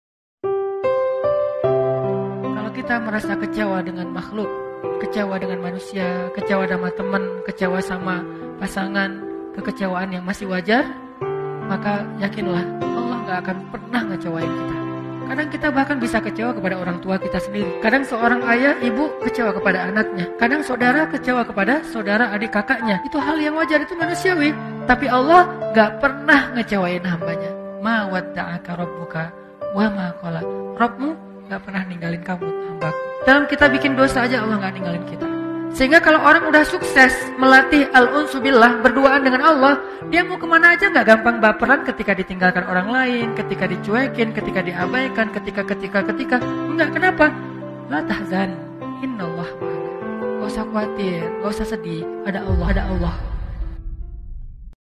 LA TAHZAN | Ustadz Hanan Attaki
LA TAHZAN INNAALLAHA MA'ANA
sound : Ustadz Hanna Attakin

📌note
jangan bersedih, LA TAHZAN, ada Allah yang mengatur segalanya

semoga bermanfaat jazakallahu jazakallahu khair 😇